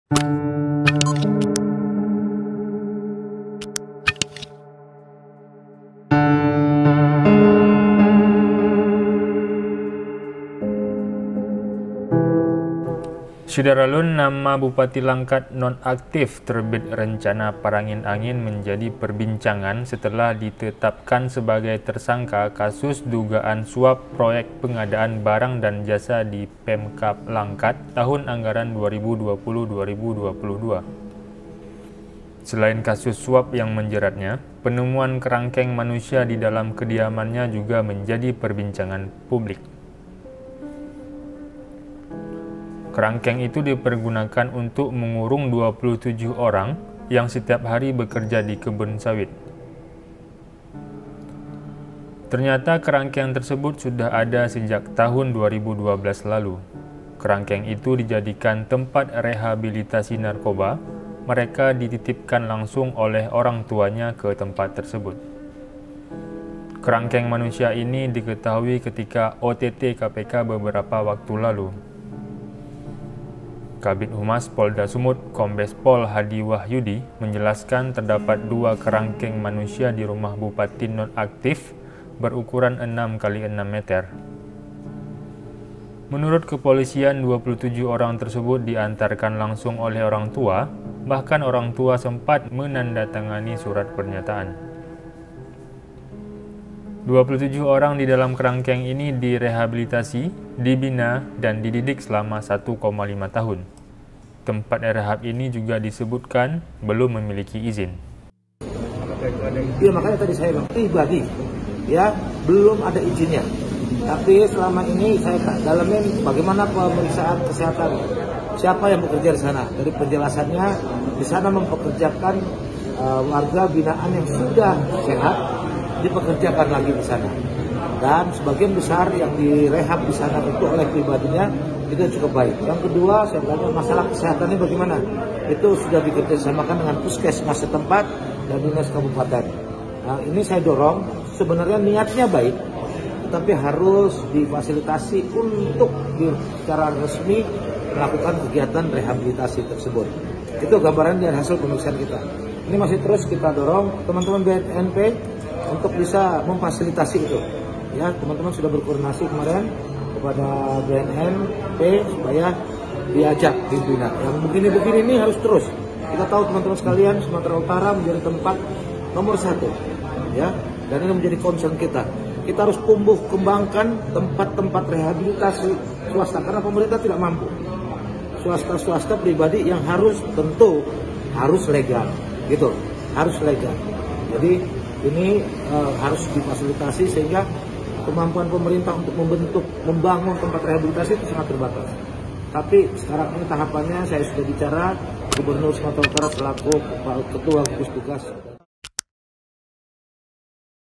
Kerangkeng Bupati Langkat Terbit Rencana Perangin Angin Ternyata Untuk Rehabilitasi Narkoba
SERAMBINEWS.COM -  Nama Bupati Langkat non aktif Terbit Rencana Perangin Angin menjadi perbincangan setelah ditetapkan sebagai tersangka kasus dugaan suap proyek pengadaan barang dan jasa di Pemkab Langkat tahun anggaran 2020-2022.

Selain kasus suap yang menjeratnya, penemuan kerangkeng manusia di dalam kediamannya juga menjadi perbincangan publik.

Kerangkeng itu dipergunakan untuk mengurung 27 orang, yang setiap hari bekerja di kebun sawit.

Mengutip dari Kompas.com, Selasa (25/1/2022) ternyata kerangkeng tersebut sudah ada sejak tahun 2012 lalu.

Kerangkeng itu dijadikan tempat rehabilitasi narkoba, mereka dititipkan oleh orang tuanya ke tempat tersebut.

Kerangkeng manusia ini diketahui ketika OTT KPK beberapa waktu lalu.

Kabid Humas Polda Sumut, Kombes Pol Hadi Wahyudi, menjelaskan terdapat dua kerangkeng manusia di rumah bupati non aktif, berukuran 6x6 meter.

Menurut kepolisian, 27 orang tersebut diantarkan langsung oleh orang tua mereka, bahkan orang tua telah menandatangani surat pernyataan.

27 orang di dalam kerangkeng ini direhabilitasi, dibina dan dididik selama 1,5 tahun.

Tempat rehab ini disebutkan belum memiliki izin.